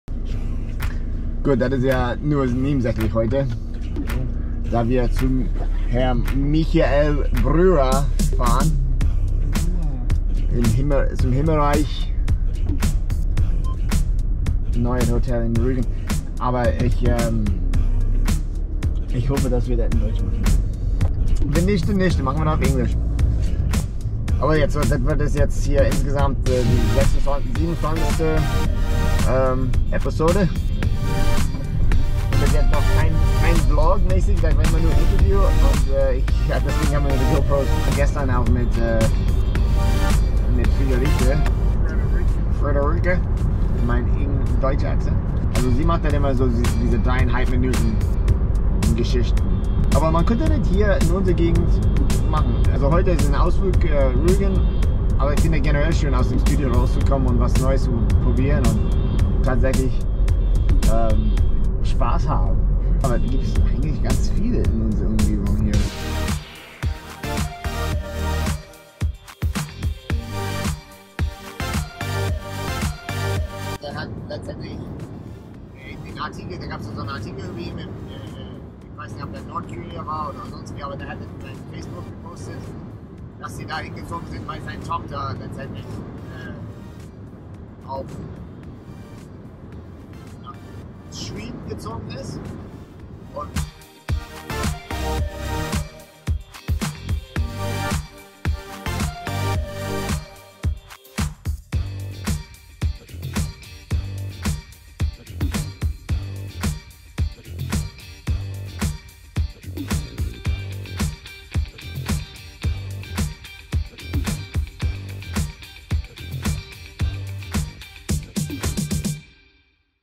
MY FIRST VLOG
Eric and I took a trip up to visit Himmelreich Rügen and Interview one of the owners Michael Brewer. Interview coming soon!